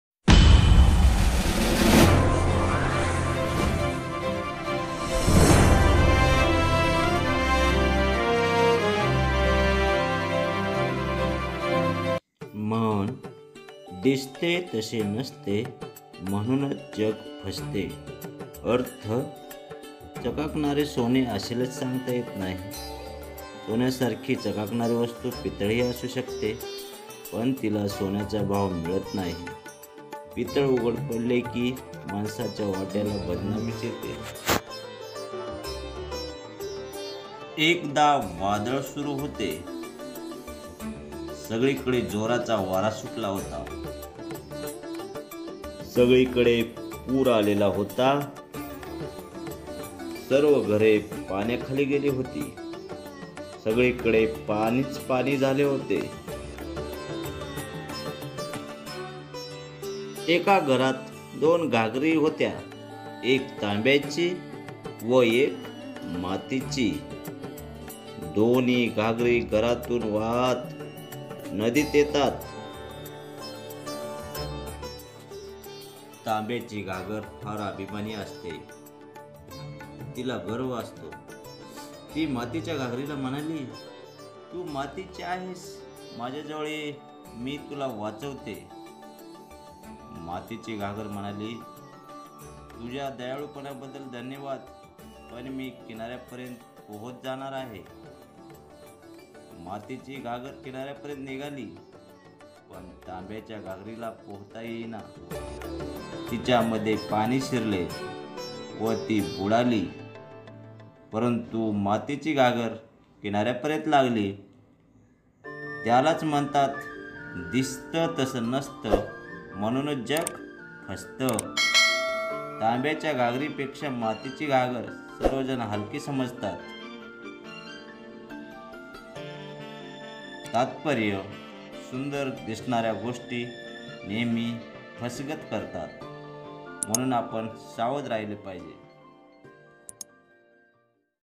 दिसते तसे नसते,म्हणूनच जग फसते.(कथारूप म्हणी)
या व्हिडिओमध्ये म्हणीवर आधारित कथेचे सादरीकरण करण्यात आले आहे.